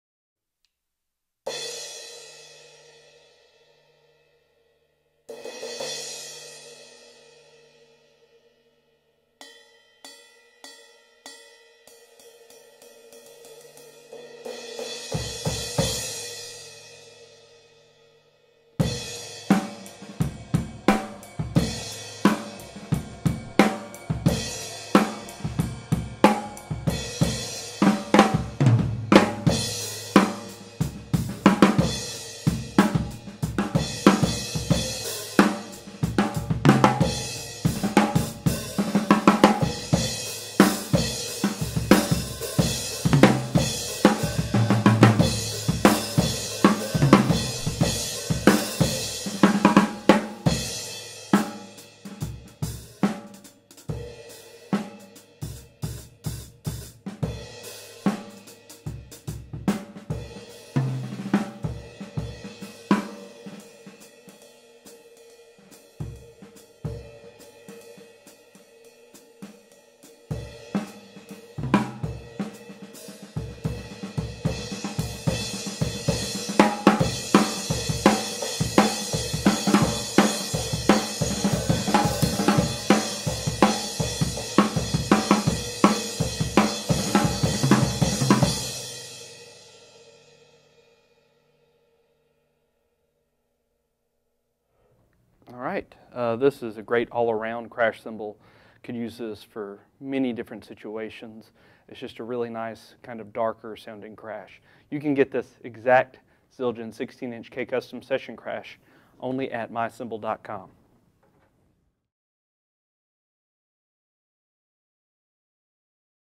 Used Zildjian 16" K Custom Session Crash Cymbal - 1060g (UK0990-1090116M)
*SOLD* Used Zildjian 16" K Custom Session Crash Cymbal.

Other gear used in this video:
• Cymbals (From drummers L to R): Zildjian 13" K Custom Dark Hi Hat Cymbals,  Zildjian 20" K Custom Ride Cymbal - Brilliant